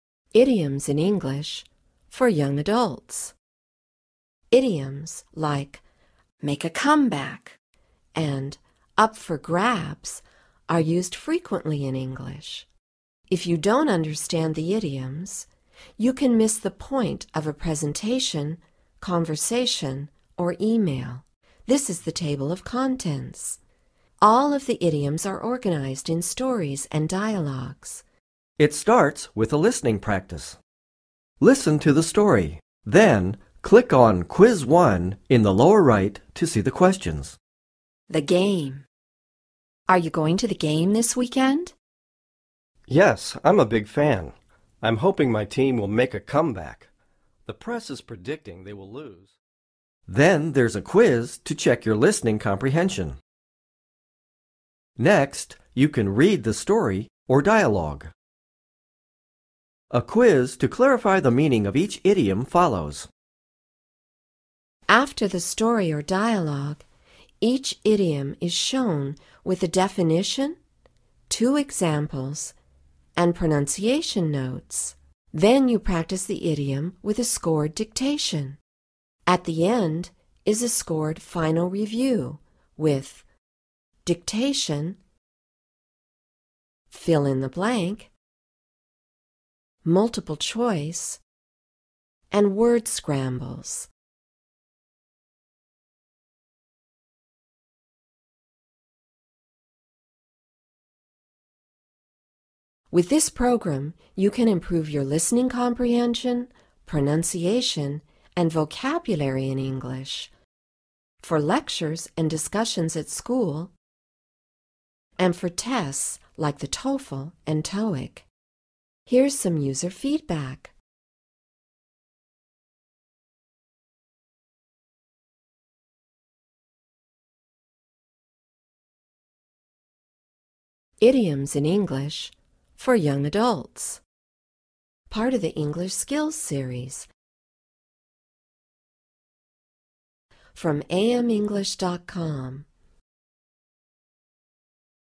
Idioms in English for Young Adults from AmEnglish.com
Idioms in English for Young Adults from AmEnglish.com. Helps build communication skills in English. Concentrates on building Listening Comprehension, pronunciation and vocabulary in English.